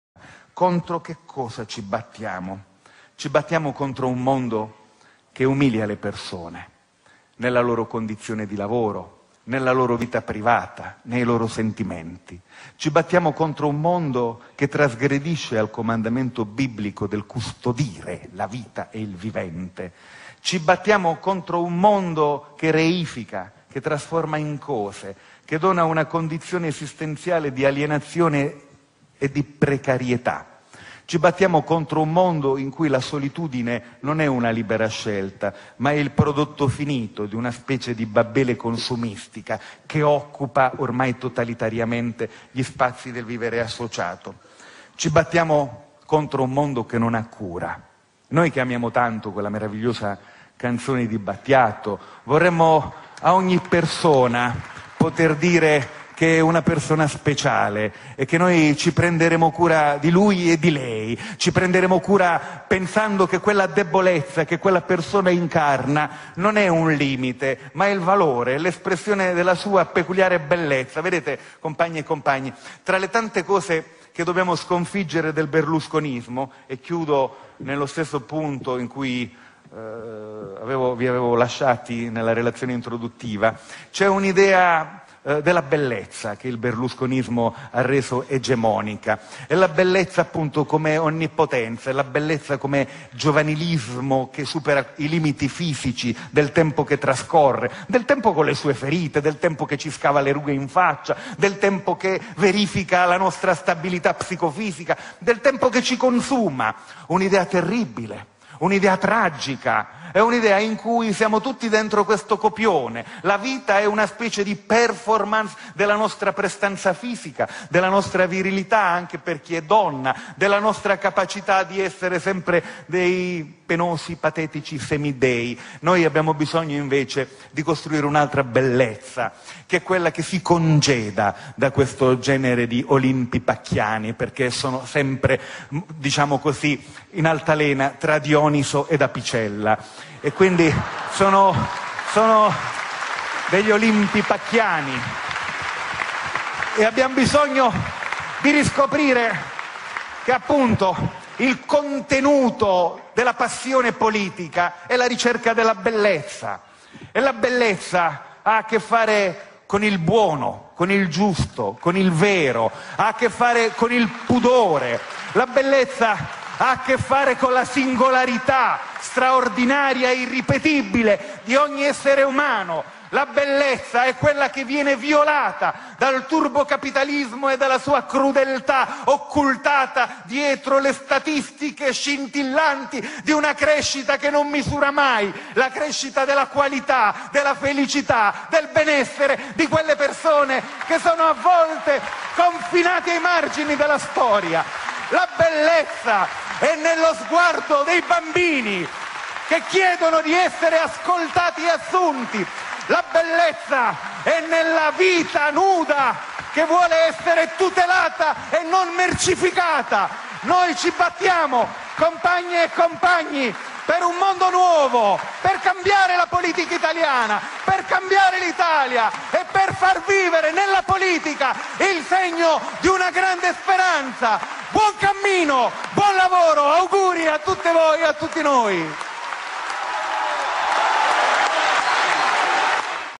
Leggendario discorso di Nichi Vendola sulla bellezza.
Uno dei migliori politici che il panorama italiano ci ha offerto e non abbiamo saputo cogliere. Una bella persona, che scrive pure da dio.